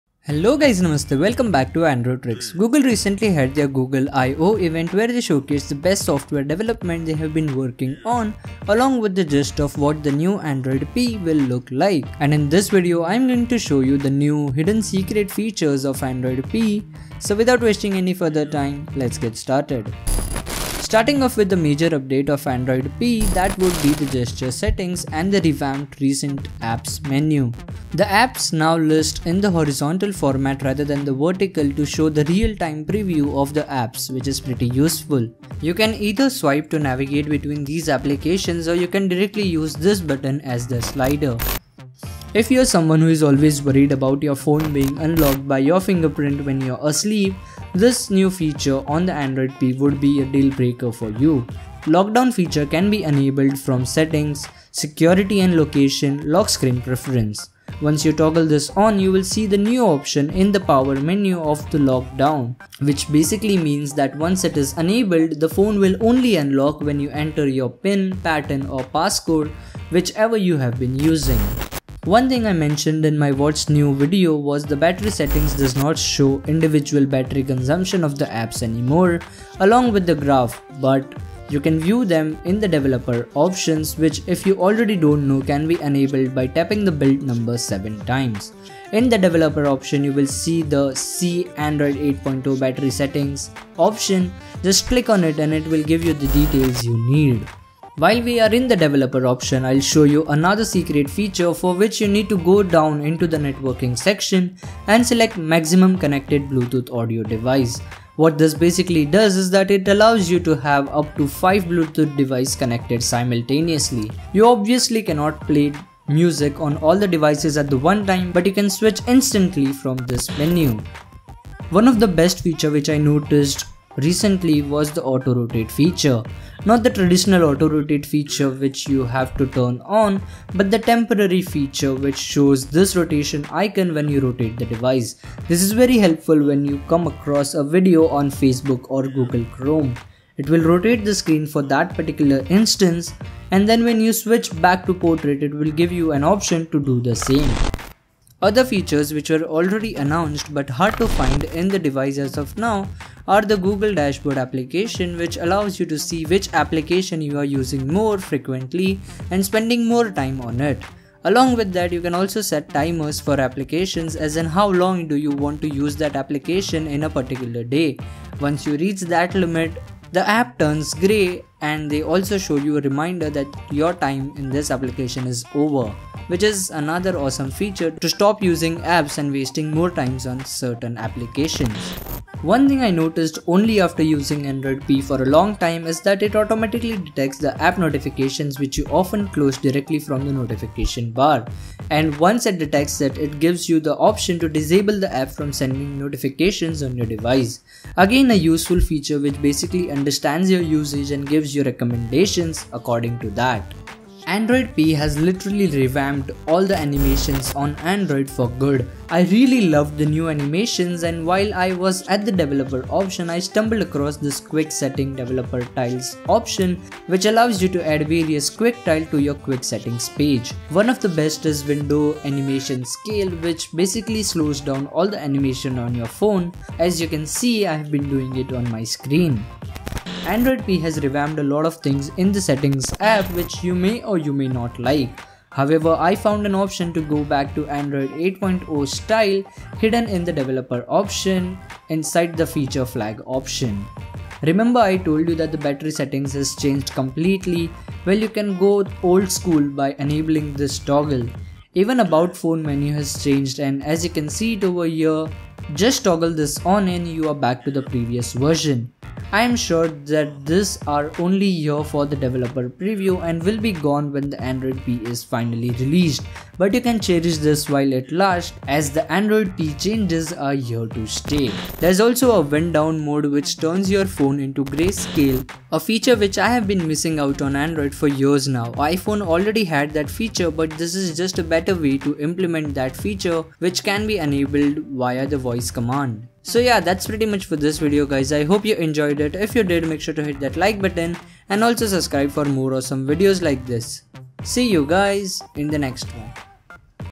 7 Outstanding Features of Android P \\ What's new in Android P?
7 New Features of Android P
What's new in Android P?

Transcript:

Hello, guys namaste welcome back to androtrix!  Google recently had their Google IO event where they showcase the best software development they have been working on along with a gist of what the next Android will be like! And in this video I'm gonna show you the new/hidden/secret features of Android P. so without wasting any time, let's get started.

Starting off with the major update of Android P would be the gesture setting and the revamped recent apps menu. The apps now list in a horizontal format rather than the verticle to show the real-time preview of the app which is pretty useful. You can either swipe to navigate between these apps or you can directly use this button as a slider.

- Lockdown
If you are someone who is always worried about your phone being unlocked by your fingerprint when you are asleep this new feature on Android P would be a deal breaker for you. Lockdown feature can be enabled from Settings-Security and location- Lock screen preference. Once you toggle this on, you will see the new option in the power button menu of lockdown which basically means that once it is enabled the phone will only unlock when you enter your PIN/Pattern or passcode whichever you have been using.

- Battery Details
One thing I mentioned in my what's new video was that battery setting doesn't show individual battery consumption of apps anymore along with the graph but you can view them in the developer option which if you already don't know can be enabled by tapping the built number 7 times. In the developer options, you will see the "See android 8.0 battery settings" just click on it and it'll give you the details you need.

- You can also connect up to 5 Bluetooth devices now on which you can see in the Media output settings.
While we are in the developer option, I will show you another secret feature for which you need to go down in the networking section and select"Maximum connected Bluetooth audio device" what this basically does is that it allows you to have up to 5 Bluetooth device connected simultaneously. You obviously cannot play from all at one time but can switch instantly from this menu.

- Auto Rotate navbar setting
One of the best feature which I noticed recently was the auto rotate feature. Not the traditional auto-rotate feature which you have to turn on but a temporary feature which shows this rotate icon when you rotate the device. this is very useful when you come across a video on facebook or chrome. It will rotate the screen for that particular instance and then when you switch back to portrait I'll give you the option to do the same.

- Hide notifications directly from the notification bar
One thing I noticed only after using the Android P for a long time is that it automatically detects the app notifications which you often close directly from the notification bar and then gives the option to disable the app to send notifications on your device. Again a useful feature which is basically understanding your usage and giving you recommendations.

- See the animation in Slow motion
Android P has literally revamped all the animations on Android for good. I really loved the new animations and while I was at the developer options I stumbled across the Quick setting developers tiles which allows you to add various quick tile to your quick settings page. One of the best is window animation scale which basically slows down all the animations on your smartphone as I have been doing on the screen.

- Feature Flags.
Android P has revamped a lot of things in the settings app which you may or may not like. However, I got found an option to go back to android 8.0 style hidden in the developer option inside the feature flags. remember I told you that the battery settings has changed completely, we'll you can go old school by enabling this toggle. even the about phone menu has changed as you can see it over here but just toggle this on and you are back to the previous version. I'm very sure that these are here only for developer preview and will be gone when the android P is finally released but you can cherish these while it lasts as the Android P changes are here to stay.